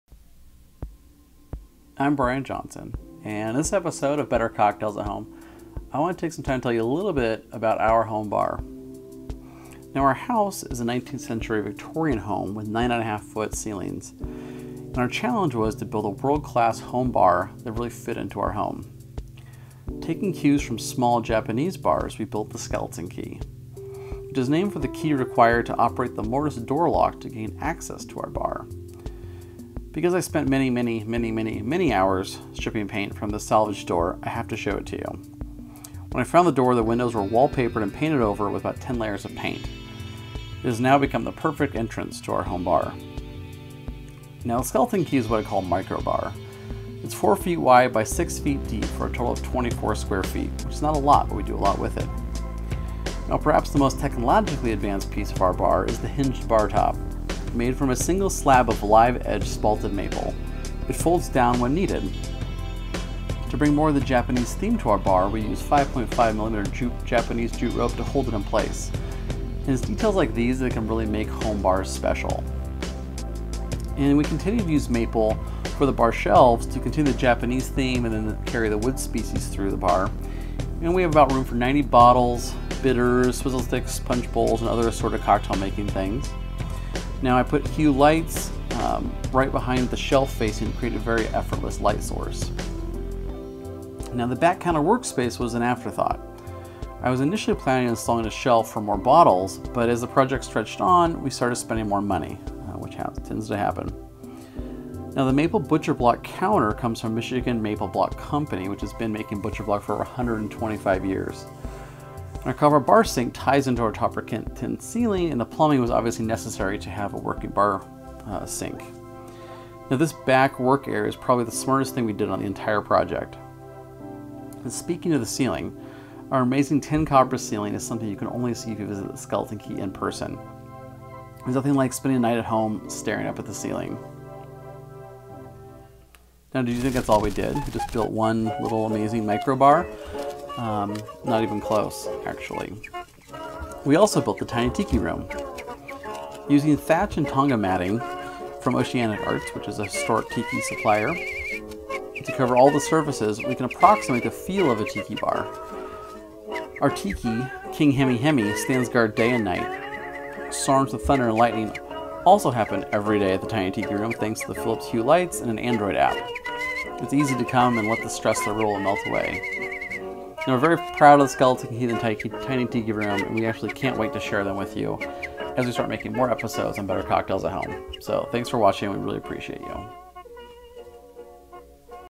The Skeleton Key and Tiny Tiki Room Home Bar Tour - Better Cocktails at Home
Hello!

Wow. It as been a very long time. Too long. But we made it. We're back!

We wanted to show you the home bar we built over the last year. The Skeleton Key and Tiny Tiki Room is a special home bar that has incorporated everything I've learned over the last decade and put it into a closet. A closet designed for cocktail making.

If you have any questions, just let me know. I'm happy to share our work.